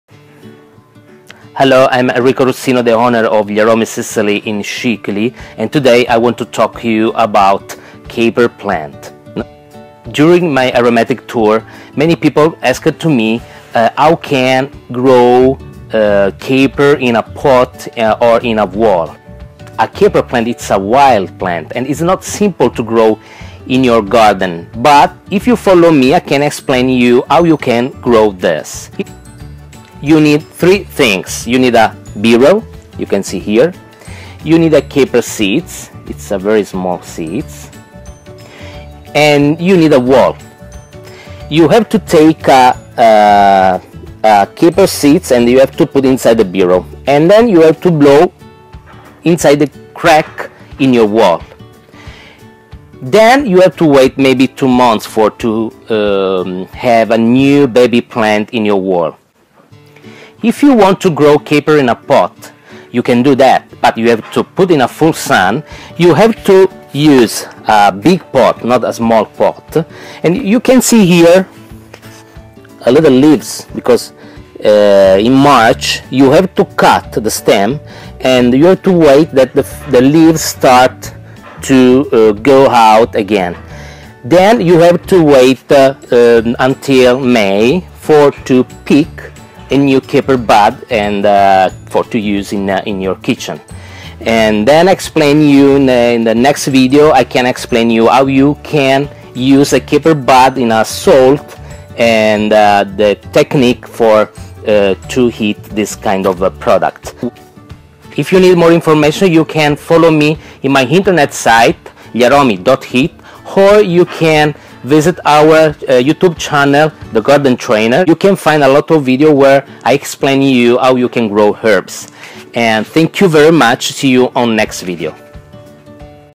Caper plant in your wall or pot
How you can grow caper plant in a wall or in a pot.
‪#‎thegardentrainer‬ ‪#‎siciliaospitalitadiffusa‬ ‪#‎sicily‬ ‪#‎montalbano‬